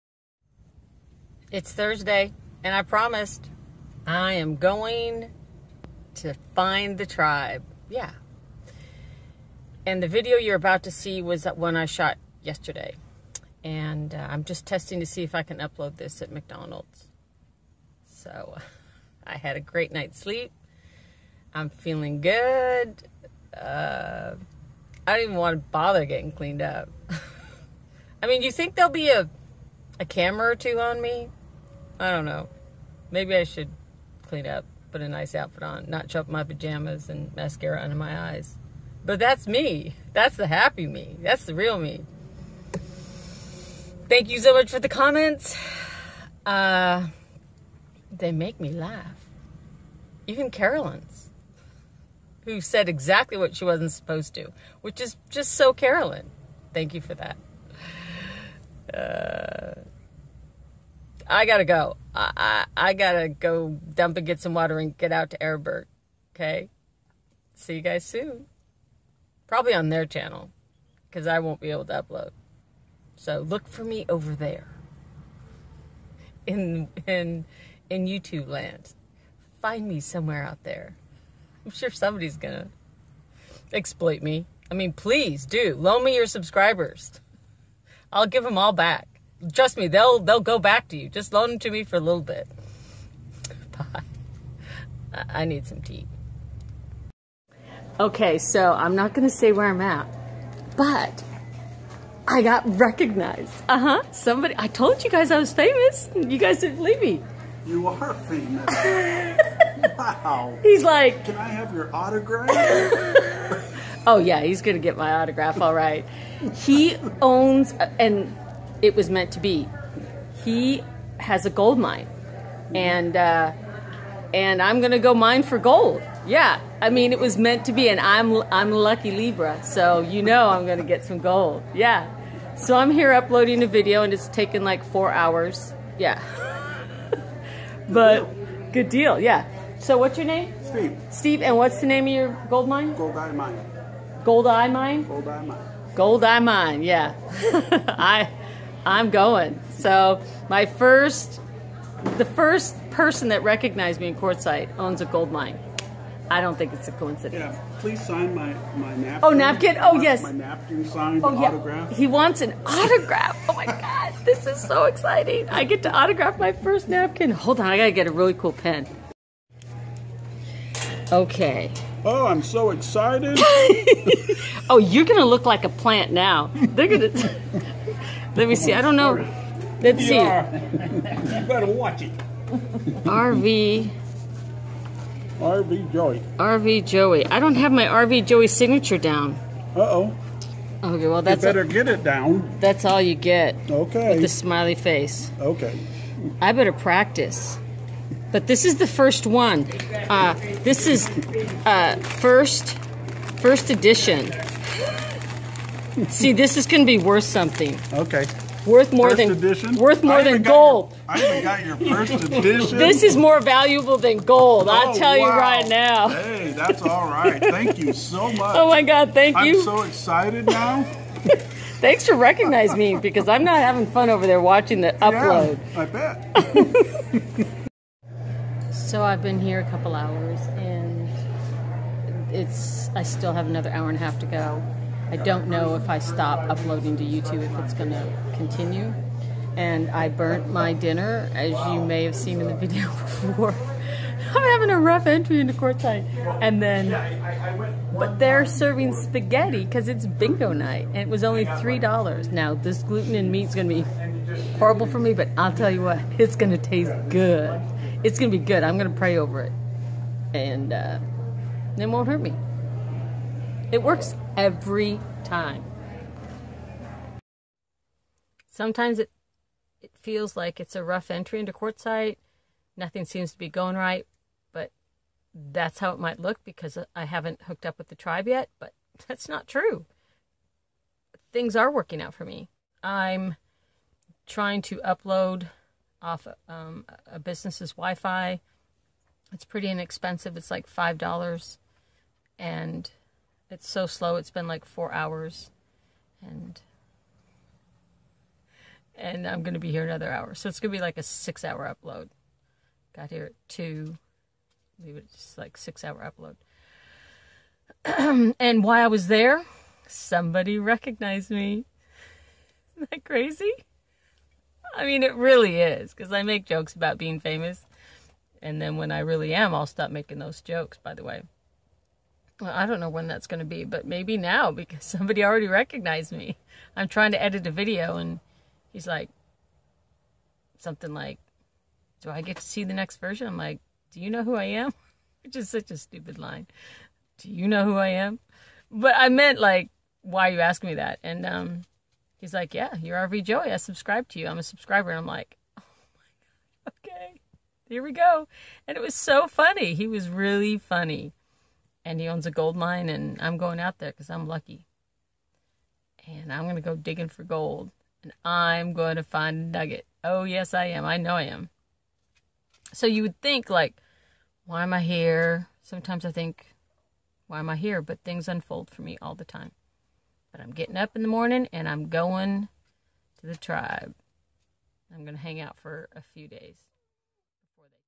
On my way to the tribe And ..A gold miner prospecting me?
Happy Thursday morning. .on my way to Tribe!  And testing my cell phone capabilities to edit and upload lower quality resolution vidoes.